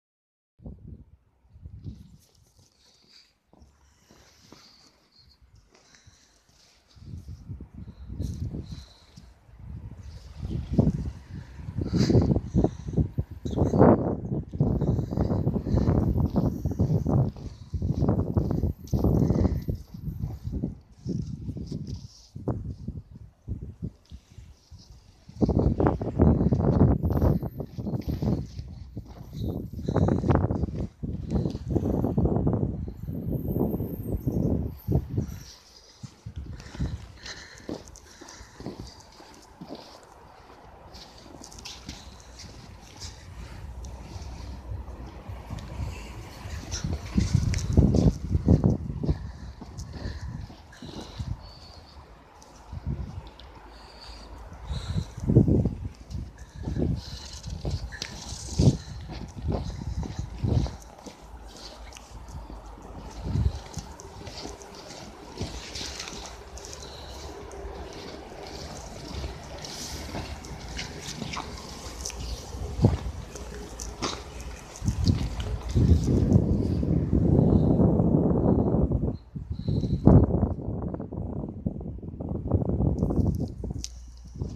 уфа 2 метровые сугробы горы снега. весной пихты снесут тракторами. проспек театр кукол 17 02 2024
город уфа 2024 г